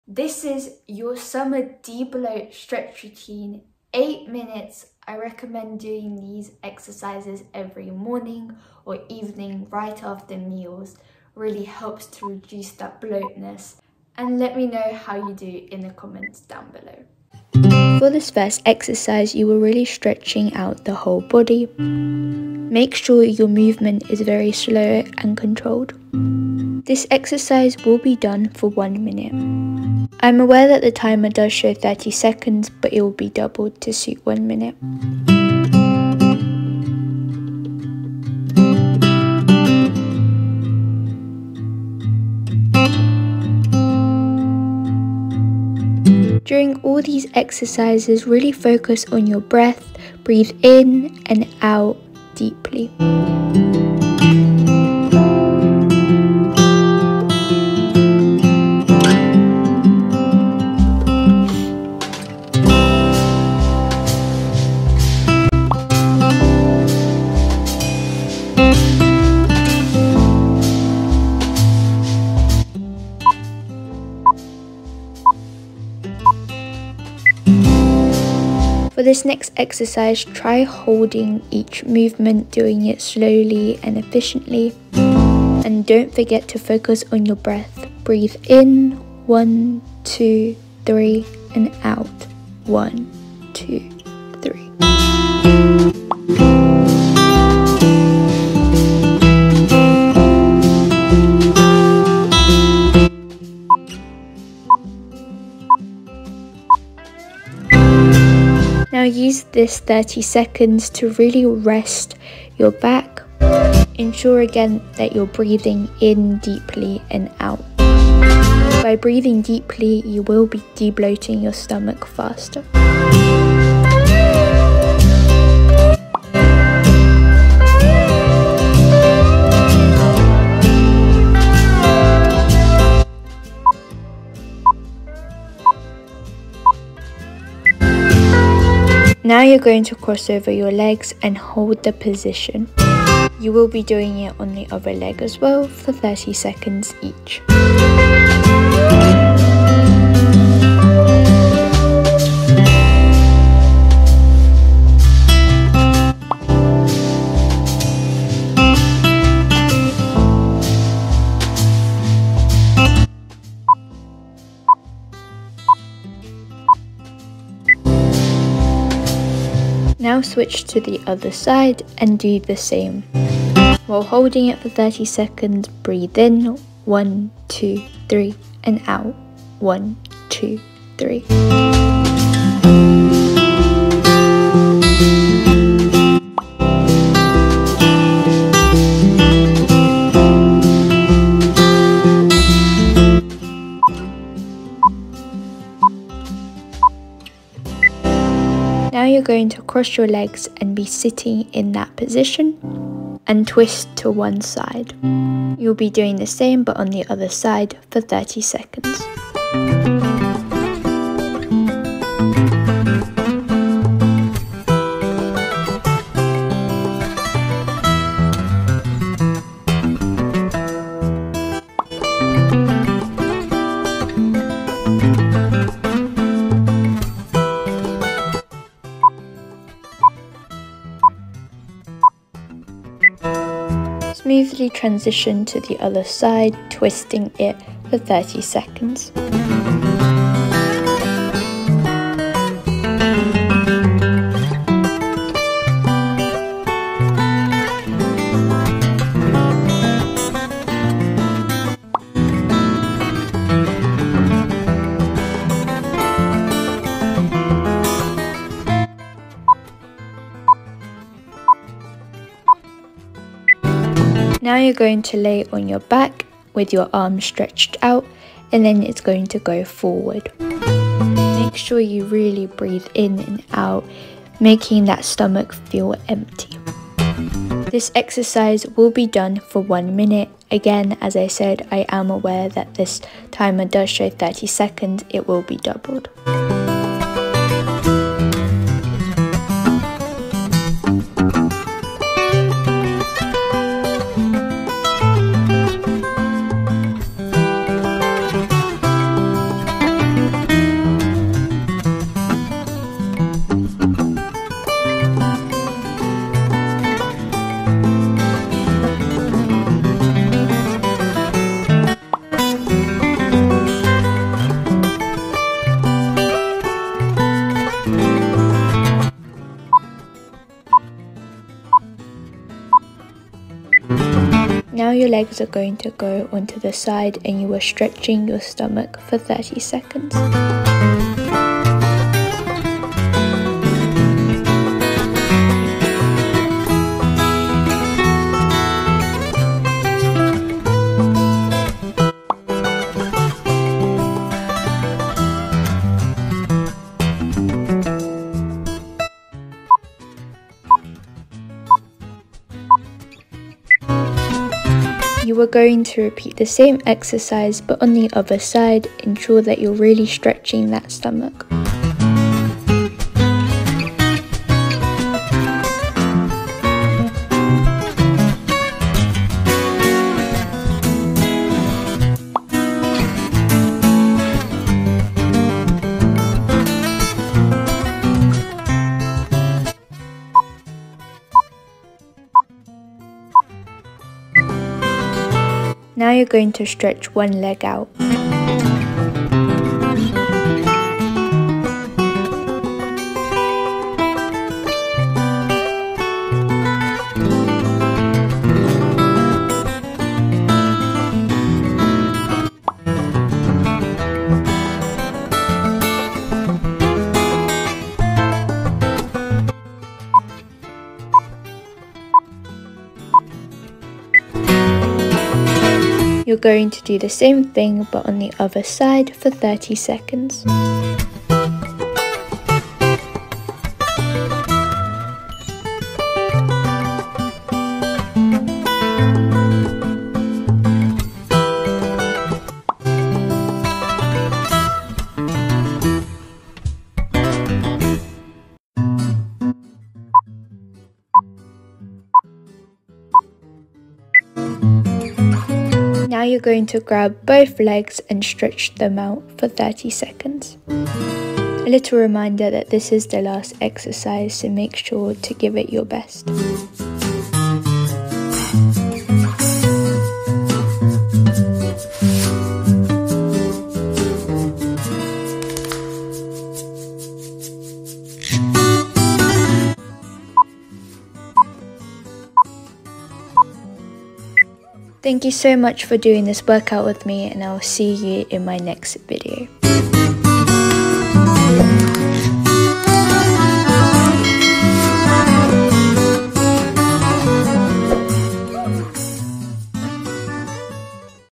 Summer DEBLOAT stomach stretch routine - 8 minutes decreases BLOATING FAST
Hello everyone,

I like many struggle with bloating especially after meals. I find these stretches very useful and would recommend doing them every morning and evening, in between meals